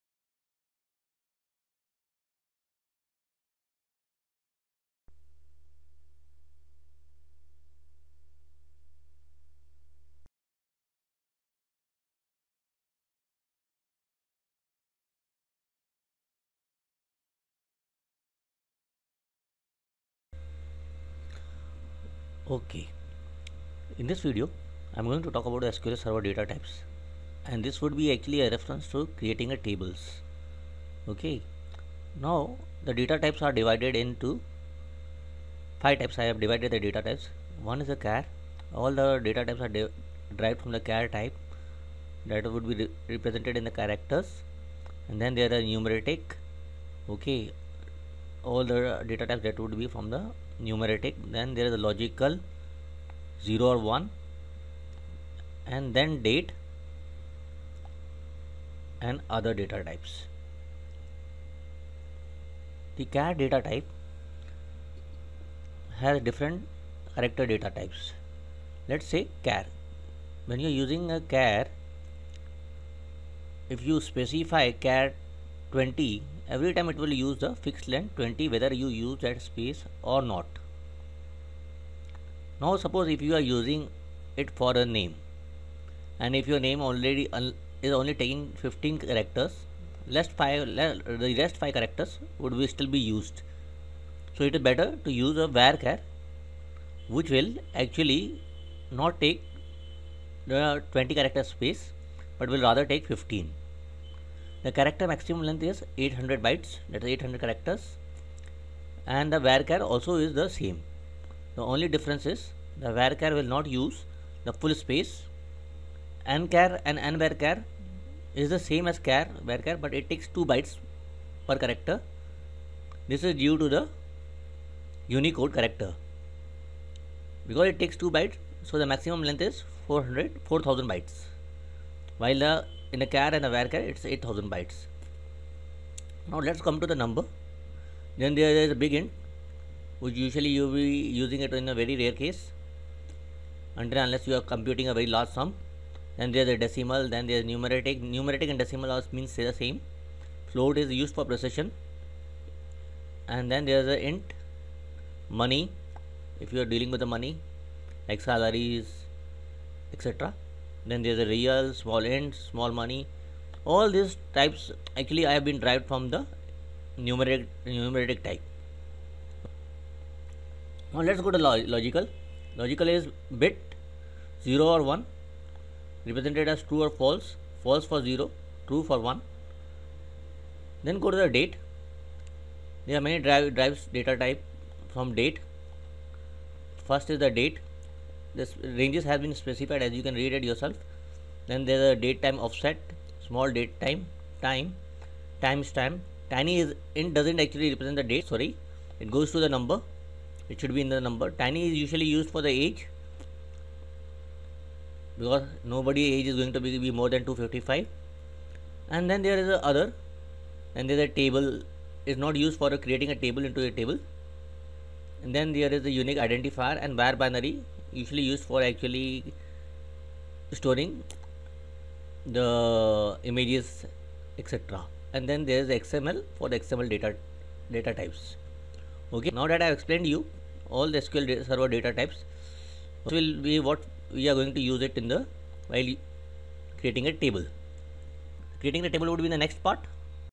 introduction to sql server 2008 data types
sql server 2008 - Data types (understand the data types before creating the tables.